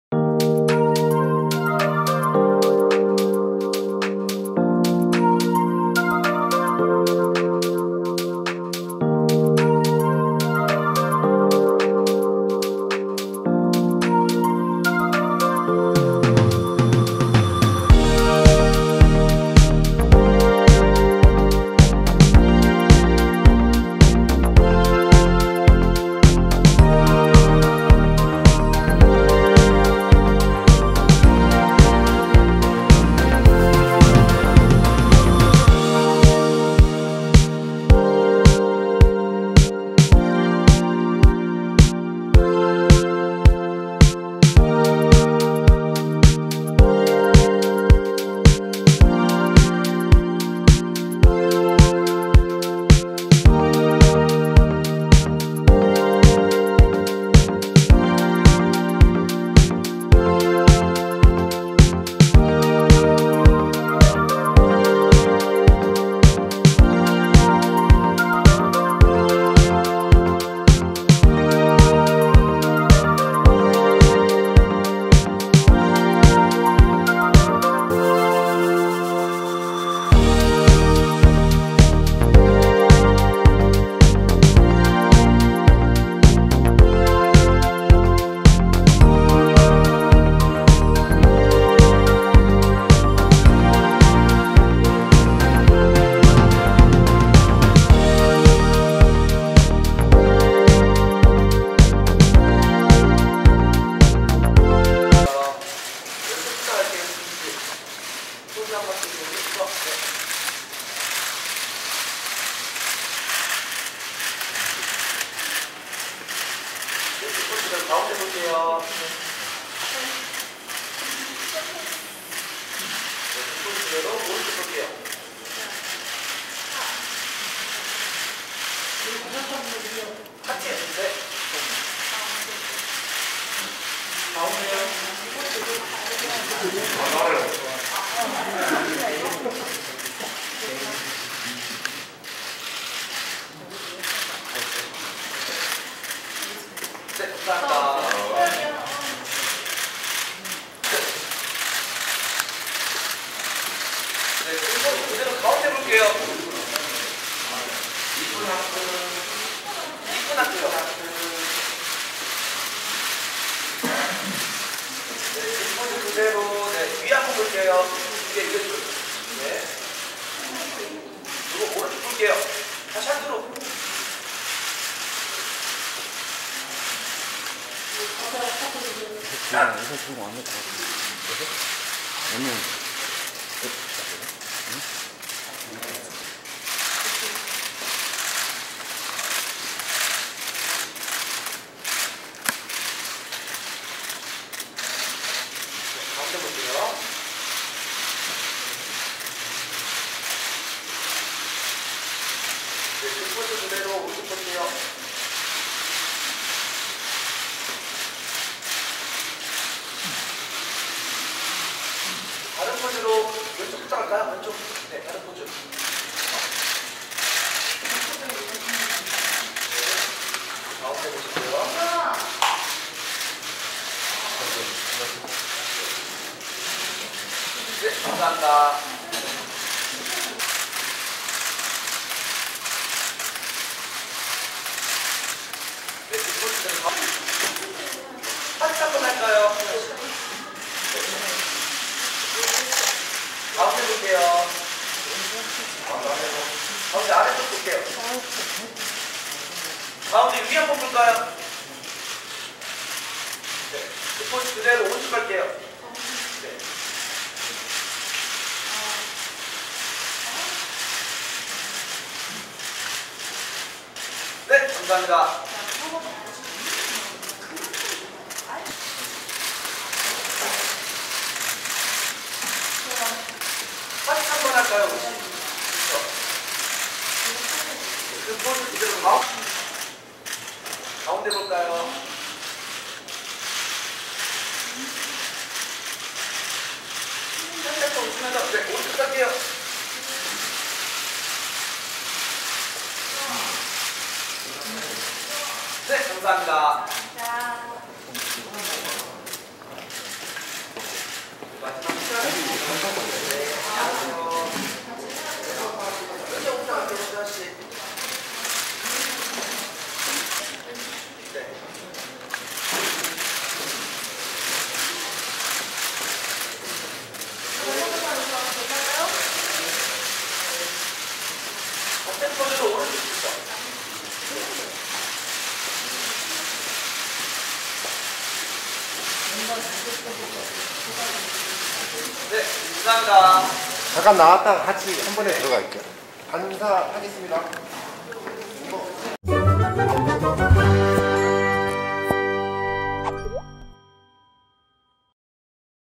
[TV텐] 'kpop 스타 게릴라인터뷰' 여자아이들, '오늘도 과즙미 장착'
[TV텐] 그룹 (여자)아이들(미연, 민니, 수진, 소연, 우기, 슈화)이 14일 오후 서울 강남구 신사동 카페 캠프통에서 열린 플레이제이 의 K-POP 스타 게릴라 인터뷰  공개녹화에 참석했다.